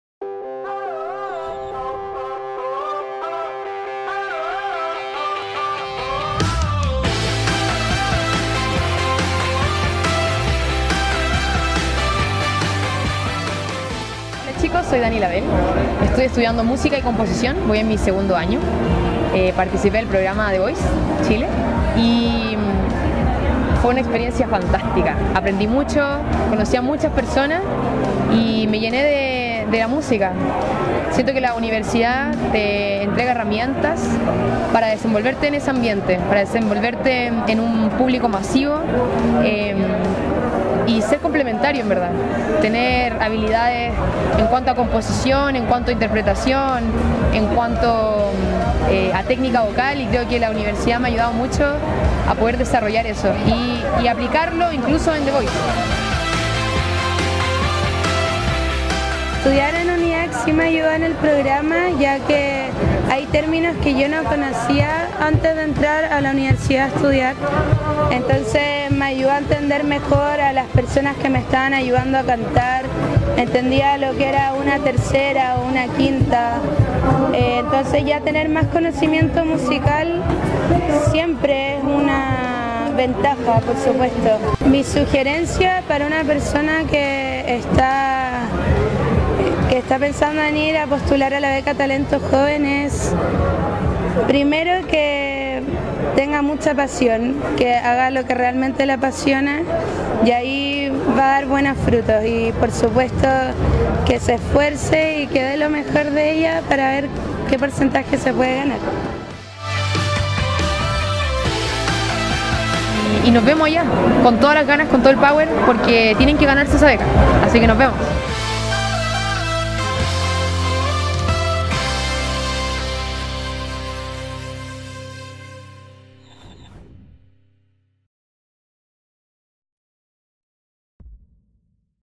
UNIACC en Feria Siad 2015
“Estudiar en UNIACC me ayudó en The Voice Chile, porque hay términos musicales que yo no conocía (…) me ayudó a entender mejor las personas que me estaban ayudando a cantar. Tener más conocimiento musical siempre es una ventaja, por eso, estoy agradecida de haber entrado a estudiar”, remarca la estudiante de Música y Composición, Consuelo Cifré.

Cifré es una de las exponentes de UNIACC en el Salón de Orientación de Alternativas Académicas, Siad, encuentro dirigido principalmente a estudiantes de enseñanza media, enfocado en las alternativas académicas, becas y formas de  financiamiento en las diferentes carreras y programas de educación superior en carreras diurnas.

En Siad -feria con más de 10 años de trayectoria- participan distintas instituciones de educación superior, como universidades, institutos profesionales, institutos de formación técnica, academias, escuelas de formación y otras entidades vinculadas con la educación.

El 28 y 29 de septiembre son las fechas para la versión de Presidente Riesco 5330, en el Parque Araucano, comuna de Las Condes. En la ocasión, Cifré y Dani Ilabel, también estudiante de UNIACC, ofrecieron su talento vocal ante los presentes.

En UNIACC, el 7 de noviembre se desarrollará una nueva versión de Beca Talento Joven, instancia en la que la universidad ofrece una beca de estudios de hasta 50% por toda la carrera, en modalidad diurna.

“Mi sugerencia para postular a  Beca Talento Joven es primero que tengan mucha pasión, que hagan lo que realmente les apasiona, y que se esfuercen”, sugiere Cifré, quien también este año fue una de las participantes en el concurso The Voice Chile.

Otra exponente de UNIACC presente en Siad y que también fue una de las protagonistas del concurso televisivo, es Dani Ilabel, estudiante de segundo año de Música y Composición.

“The Voice Chile fue una experiencia fantástica, aprendí mucho y conocí a muchas personas”, destaca la estudiante.

“Siento que la universidad entrega herramientas para desenvolverte en ese ambiente, en un público masivo. Y también para ser complementario, para tener habilidades en cuanto a composición, interpretación y técnica vocal. La universidad me ha ayudado mucho a poder desarrollar todo esto y aplicarlo en The Voice”, resume Ilabel.